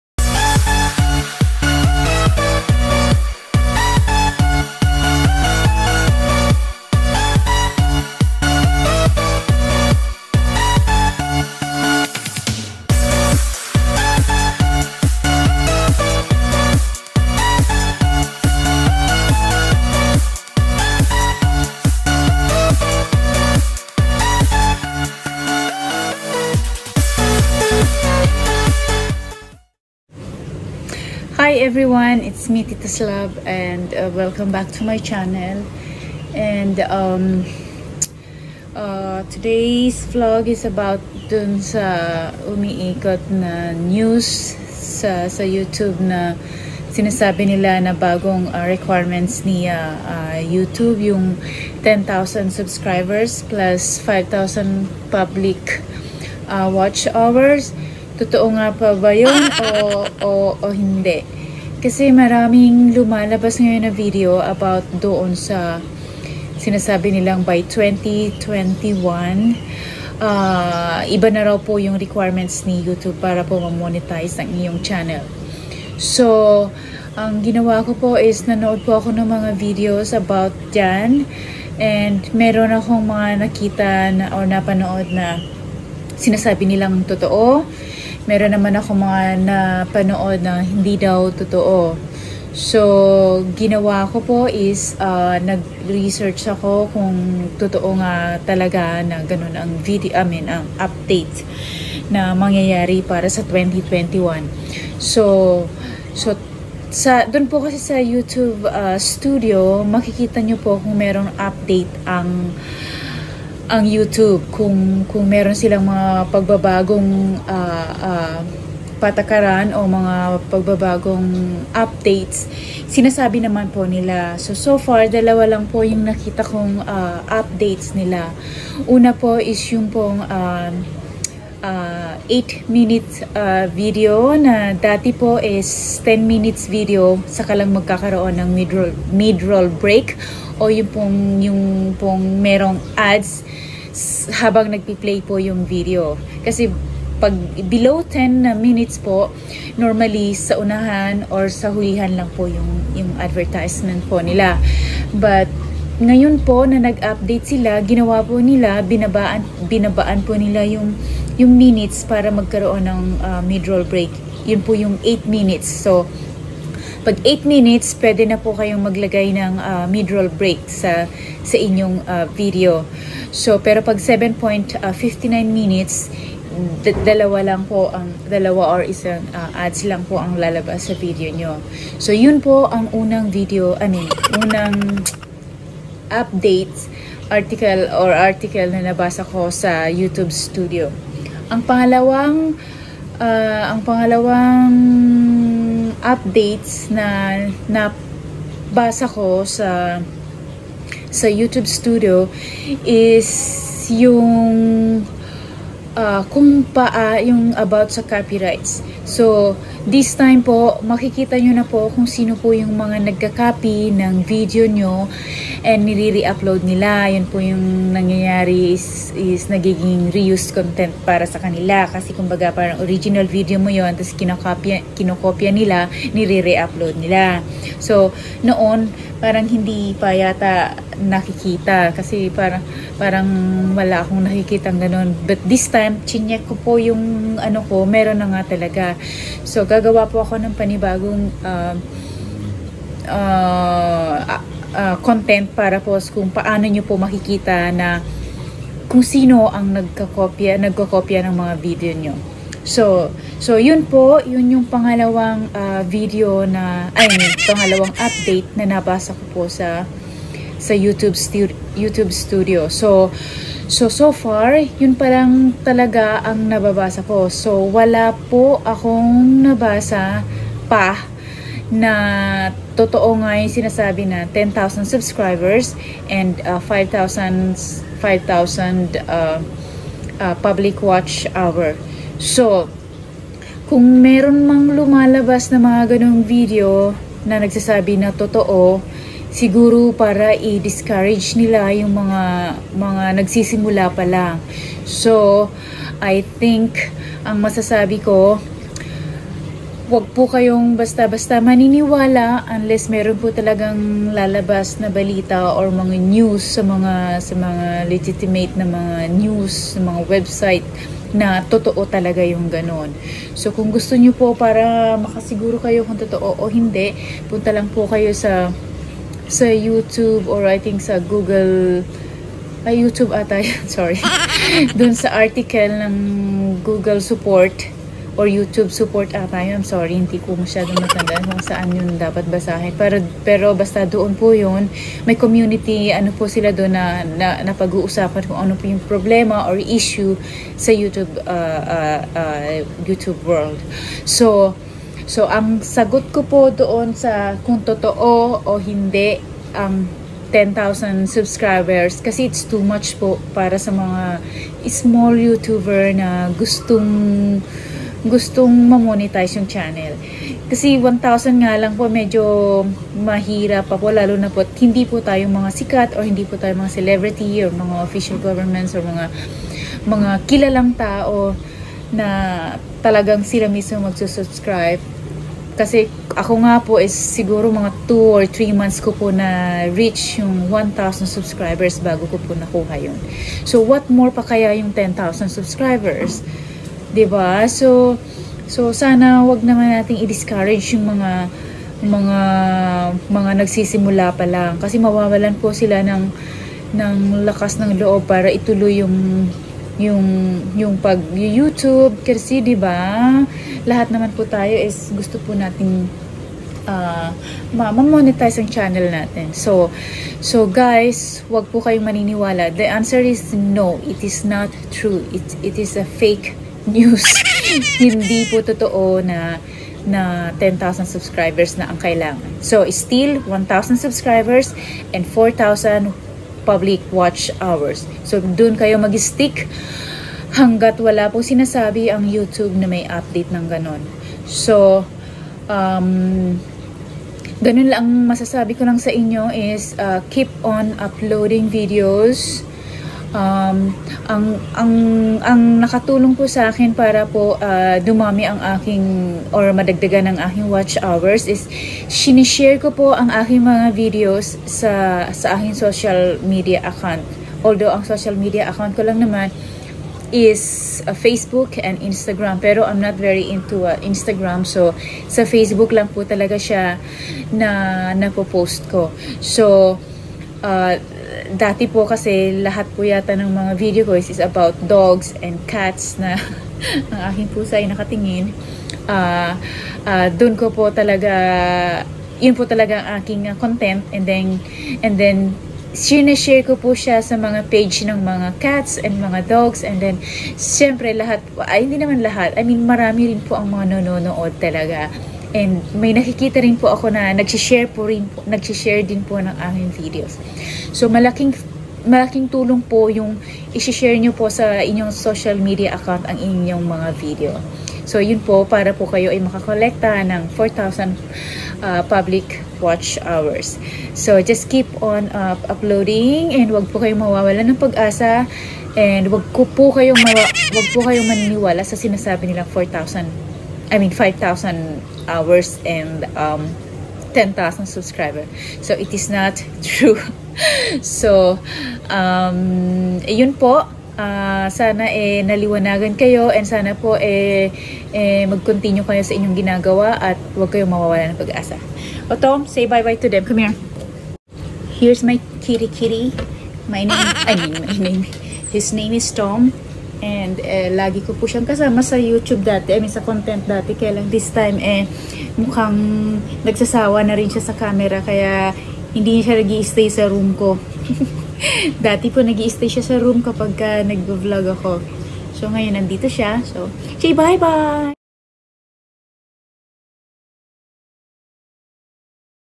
10k subs and 5k WH requirements, totoo ba?
Totoo kaya ang balita sa bagong requirements ng YouTube? Alamin natin😊